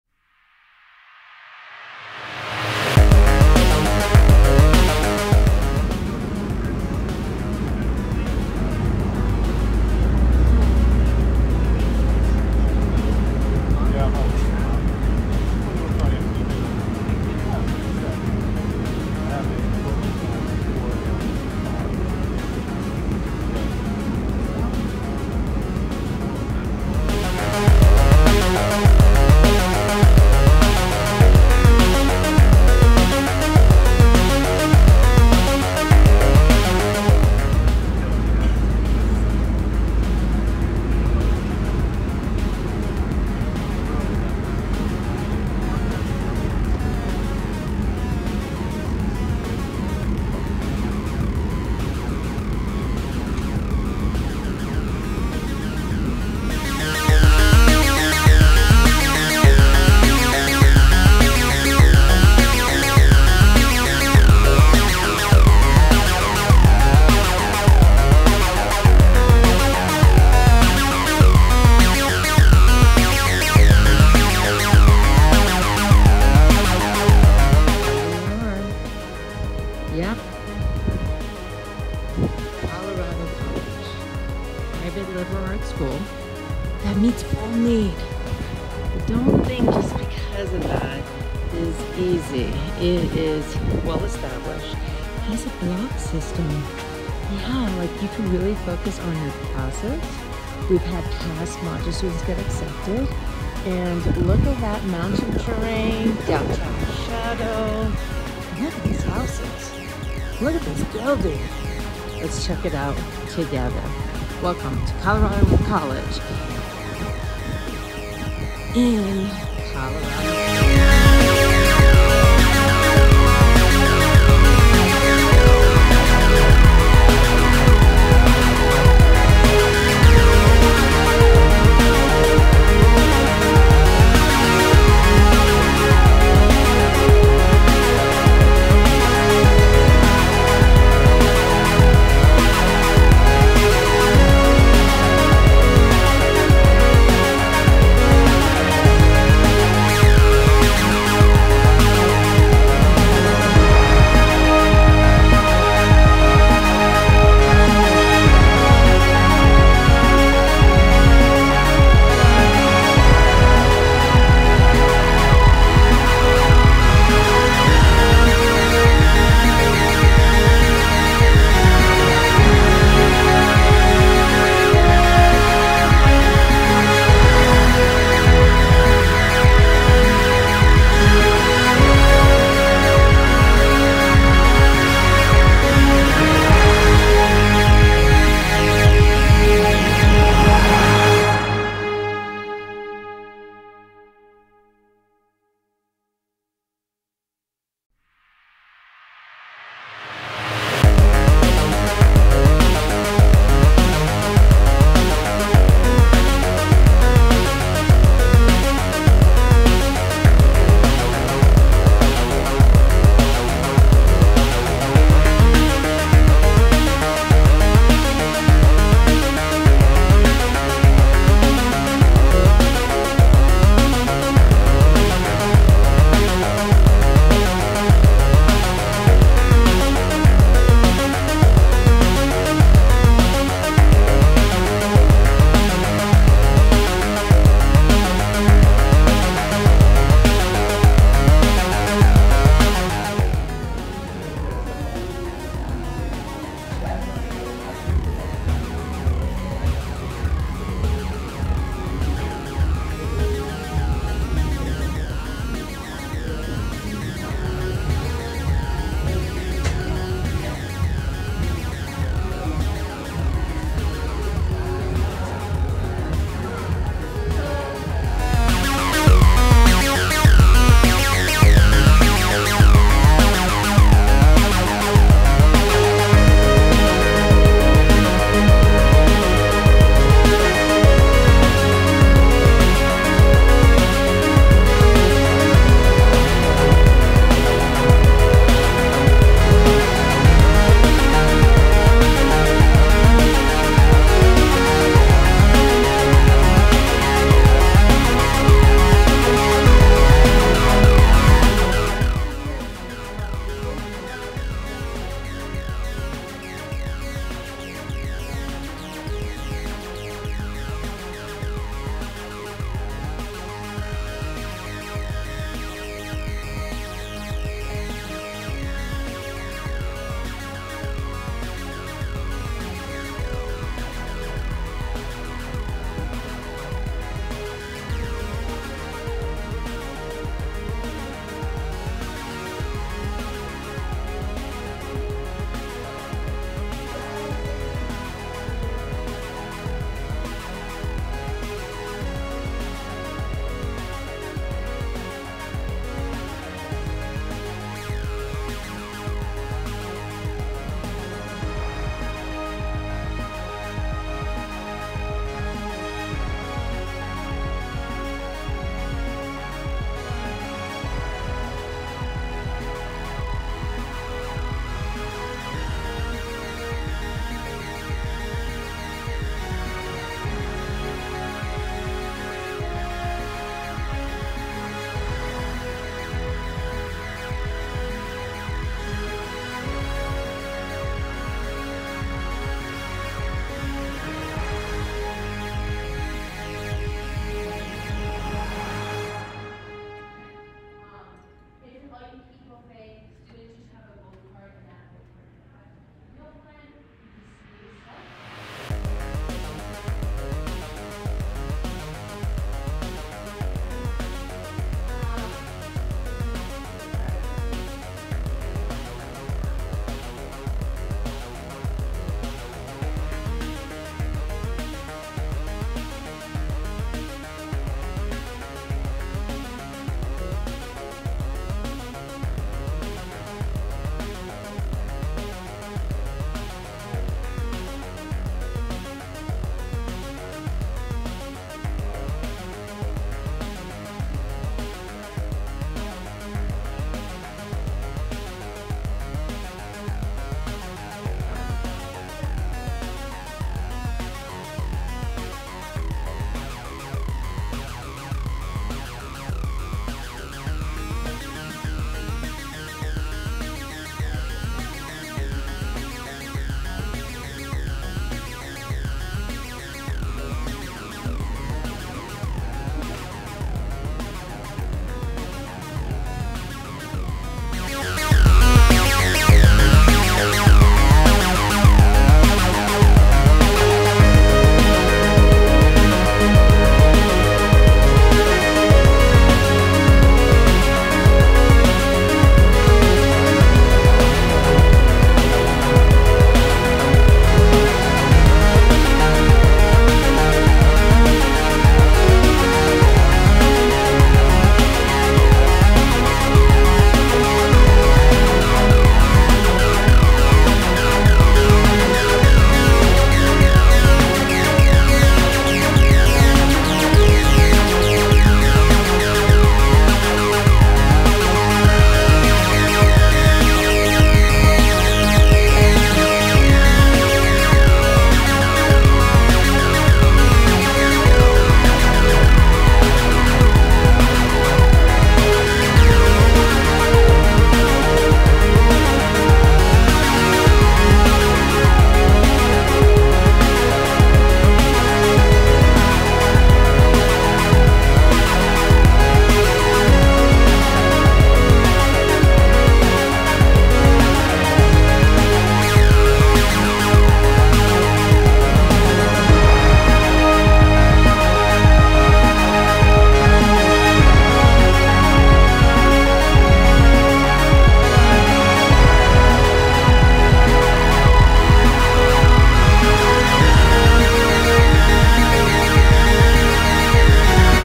Colorado College - A campus filled with opportunities
Welcome to Colorado College in Colorado Springs, Denver where Division I women’s soccer and hockey is also met with Division III major sports. It is a campus filled with new and historical structures and forms with lounges to play, cool and even barbecue. This campus meets full need but offers several support services along with opportunities for their students beyond Colorado College. They have 3.5 Block plan where you can dive into your course at a time where you have several moments of discussions, papers, readings and debates in an intimate classroom setting that beats any lecture style classroom. You know a school is good if their graduation and retention rate is competitive with a campus like Stanford. Embrace the diversity of ideas and opportunities at Colorado College! Go tigers!